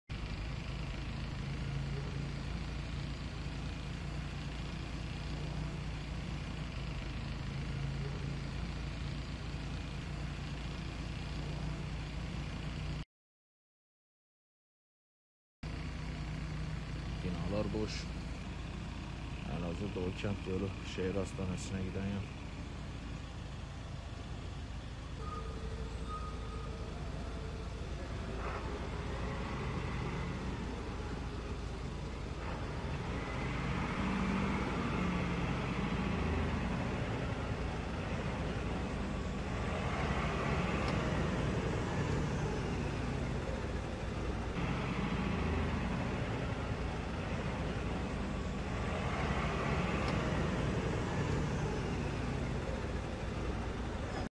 Elazığ'da Bir Binanın Çatısı Alev Alev Yandı
Elazığ'da depremde boşaltılan bir binanın çatısı alev alev yandı. İtfaiye ekipleri yangını diğer binalara sıçramadan söndürdü.